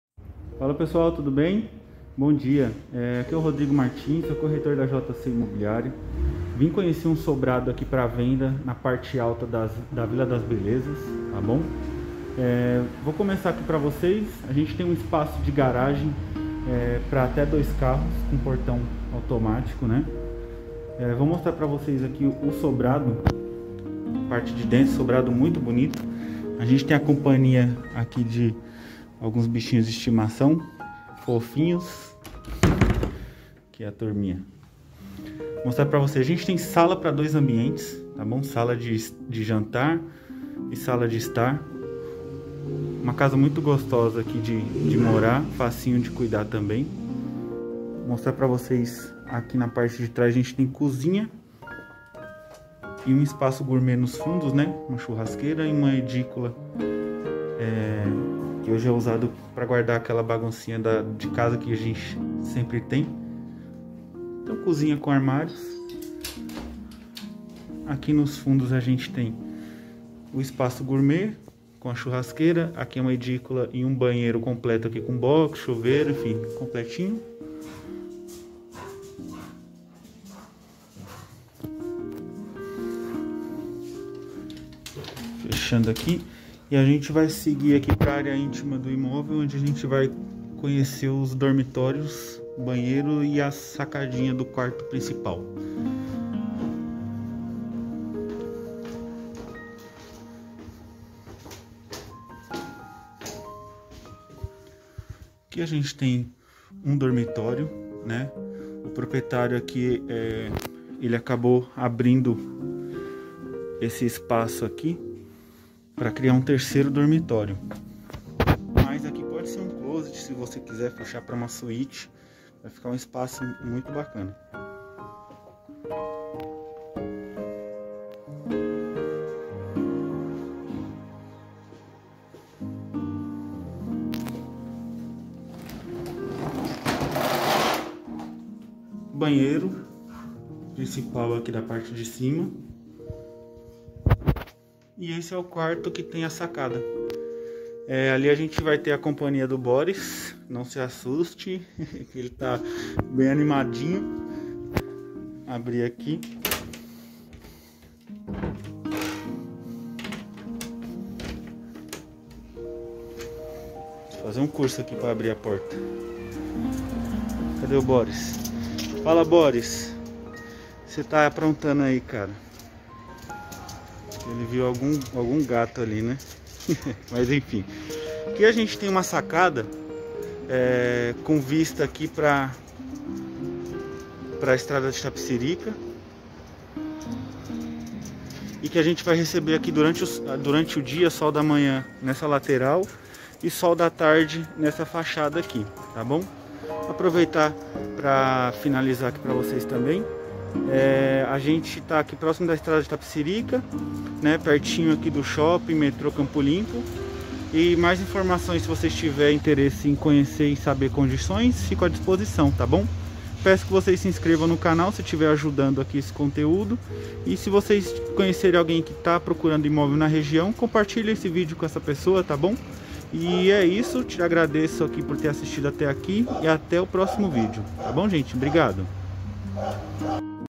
Sobrado a venda com 3 dormitórios e 2 vagas de garagem parte alta da Vila das Belezas
Lindo Sobrado com 3 dormitórios, sala ampla e aconchegante para dois ambientes em piso laminado impecável, seguimos com um lavabo, cozinha com armários e nos fundos espaço gourmet e uma edícula com banheiro completo. Seguindo para a área intima do imóvel teremos os 3 dormitórios mais 1 banheiro e no quarto principal uma sacada em "L" com vista para o bairro. o imóveis recebe sol da manha e sou da tarde e ainda dispõe de 2 vagas de garagem cobertas com portão automático. Gostou desse sobrado incrível ? Então agende uma visita com um de nossos corretores e venha conhecer uma das melhores opções do bairro.

Facebook Rodrigo Martins corretor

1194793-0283
Rodrigo Martins